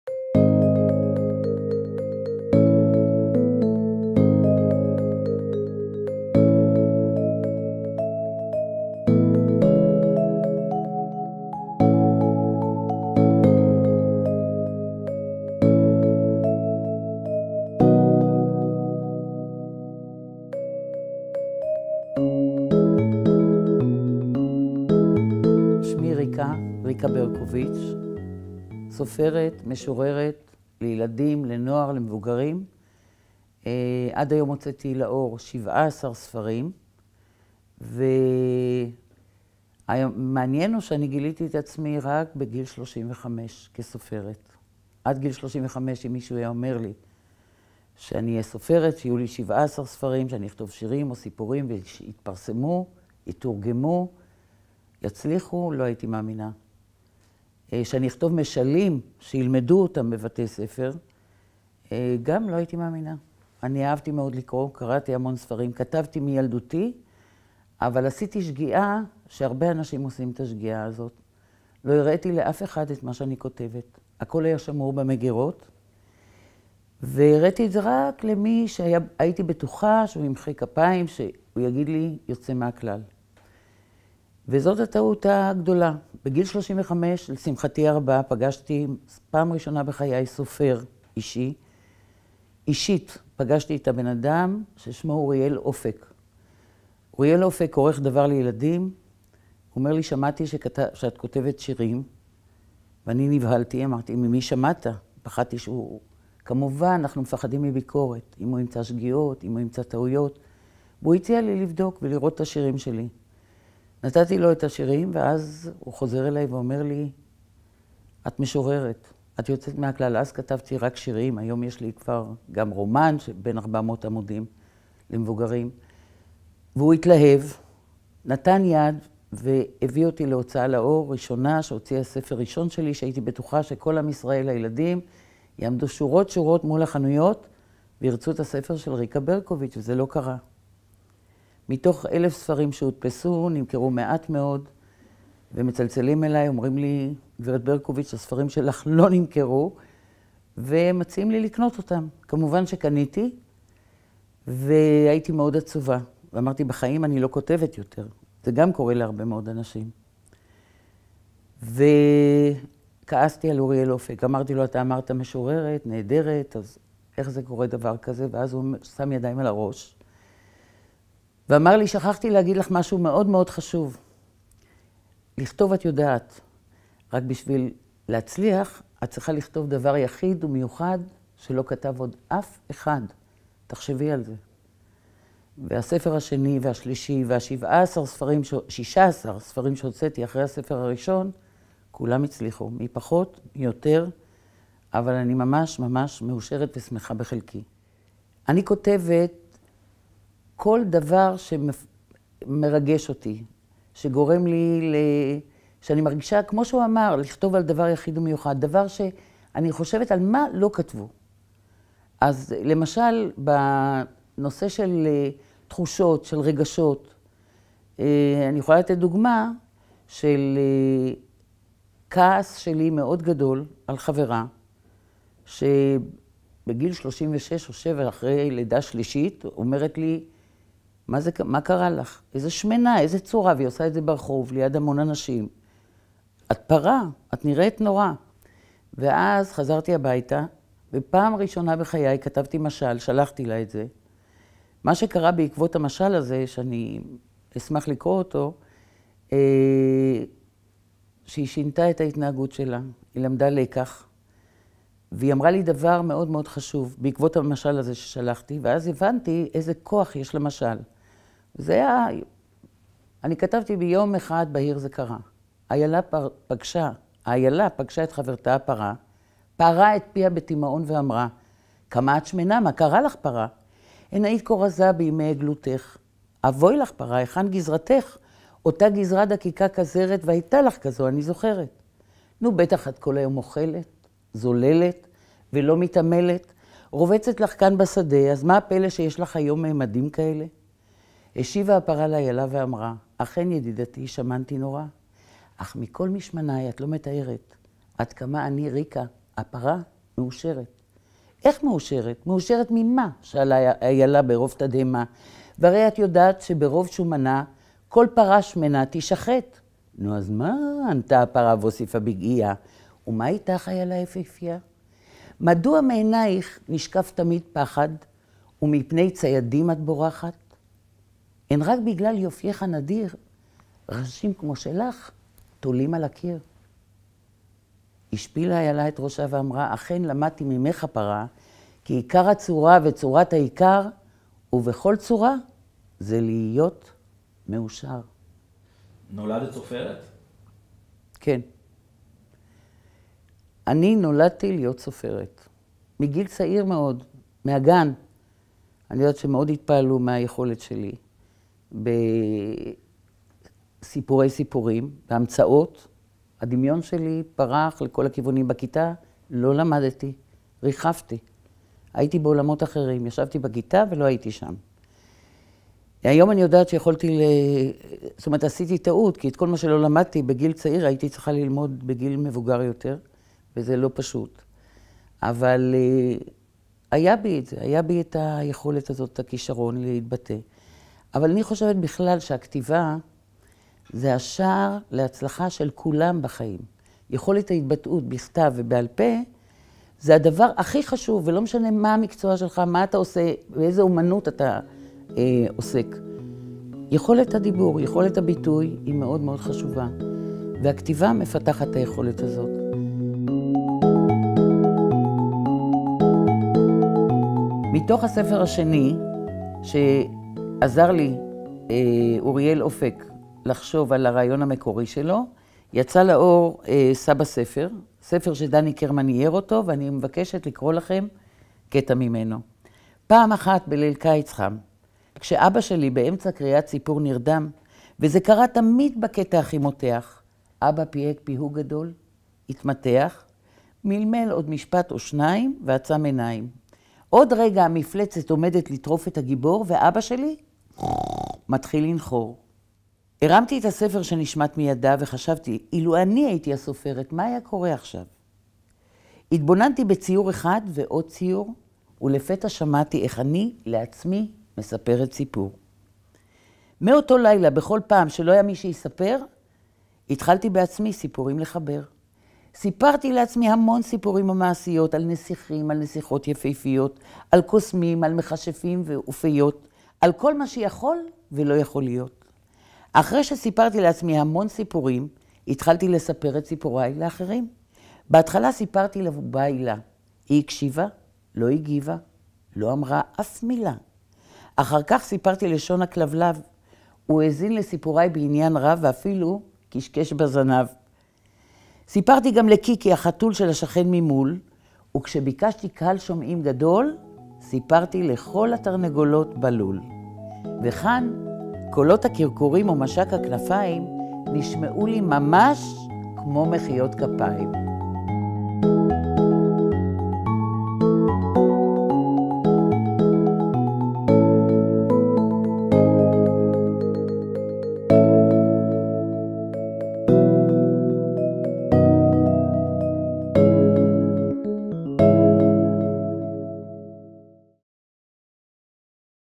"סופרים קוראים" - ריקה ברקוביץ
ריקה ברקוביץ - מתוך פרויקט "סופרים קוראים"
"סופרים קוראים" - ארכיון הווידאו של כלל סופרי ישראל.
הארכיון הוקם ביוזמת מרכז הספר והספריות, מפעל הפיס ובית אריאלה - ספריית שער-ציון.